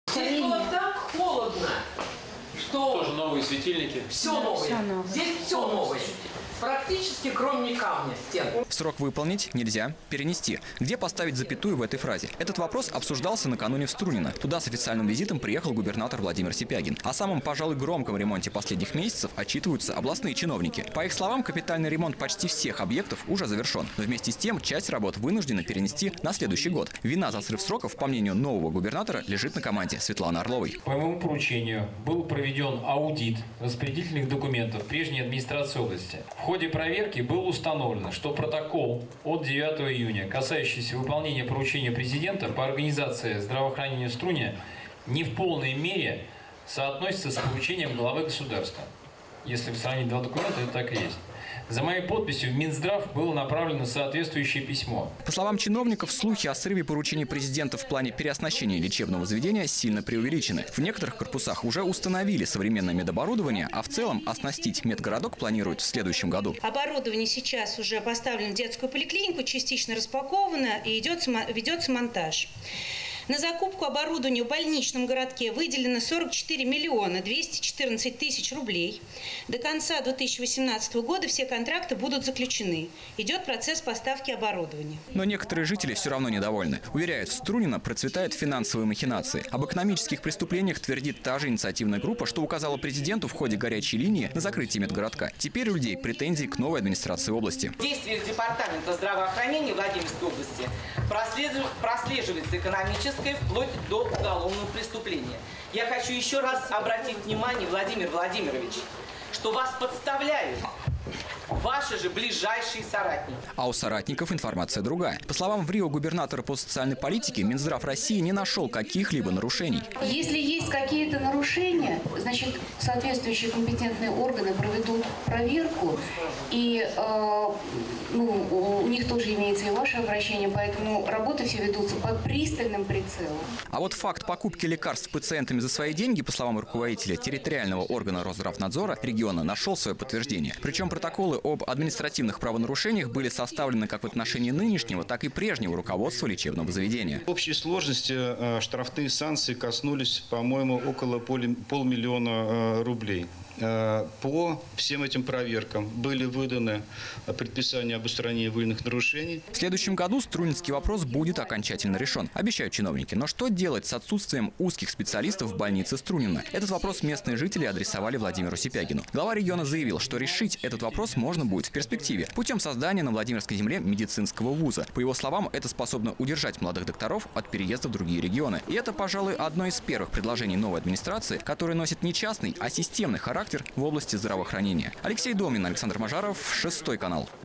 Страсти по Струнино
И снова Струнино. Губернатор области Владимир Сипягин накануне провел там почти весь день. Глава региона встретился с общественностью и провел на месте заседание рабочей группы по контролю за исполнением поручения Президента России. Напомним, "струнинская" тема прозвучала во время прямой линии с главой государства. Жители были обеспокоены закрытием медучреждения. Готовы ли власти выполнить поручение в срок, который уже на исходе? Знает Алексей Домнин.